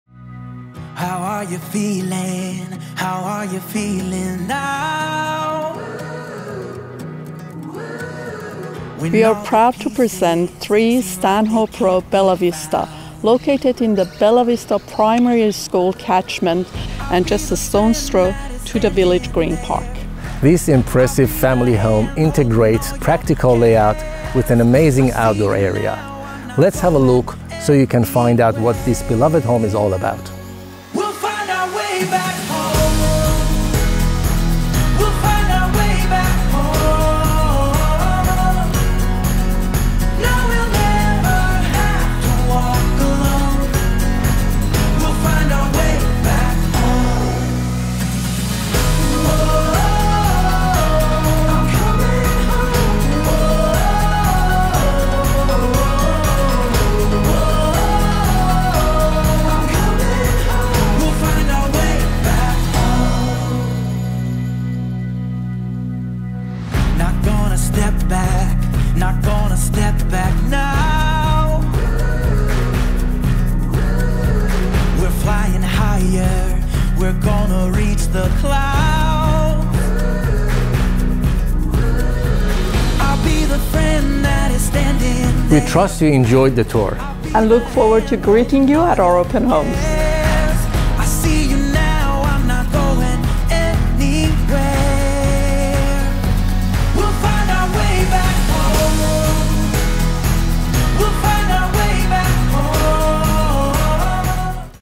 3 Stanhope Row, Bella Vista
Discover the perfect blend of comfort and practicality in this inviting property, nestled within the sought after Bella Vista Public School catchment. Located on a tree-lined whisper quiet cul de sac, this beloved home features a very spacious backyard, ideal for family gatherings and entertaining guests. The massive alfresco area provides a shaded retreat, while the sparkling swimming pool invites endless summer fun and relaxation.